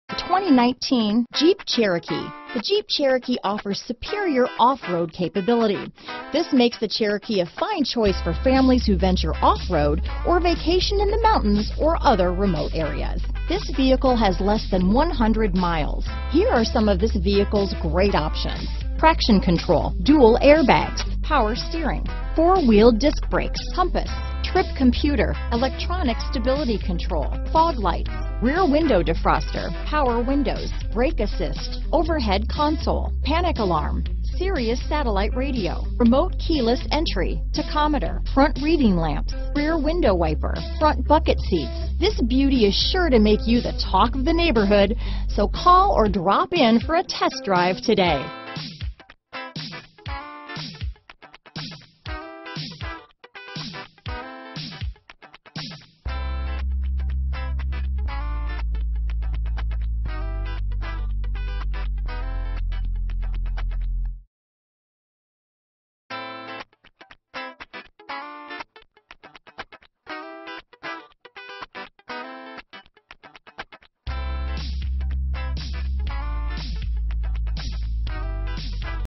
2019 Jeep Cherokee Tyler, Longview, Mineola, Jacksonville, Lindale, TX C420225
Diamond Black N 2019 Jeep Cherokee available in Tyler, Texas at Patterson CDJR Hyundai Tyler. Servicing the Longview, Mineola, Jacksonville, Lindale, TX area.
2019 Jeep Cherokee  - Stock#: C420225 - VIN#: 1C4PJLCBXKD420225

3120 S SW Loop 323

$4,000 off MSRP!brbr2019 Jeep Cherokee Latitude FWD 9-Speed 948TE Automatic 2.4L I4 Diamond Blackbrbr22/31 City/Highway MPG Price includes: $1,000 - Chrysler Capital 2019 Bonus Cash **CK5 (CA,MA,SE,SW). Exp. 04/30/2019, $2,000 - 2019 Retail Consumer Cash 63CK1. Exp. 04/30/2019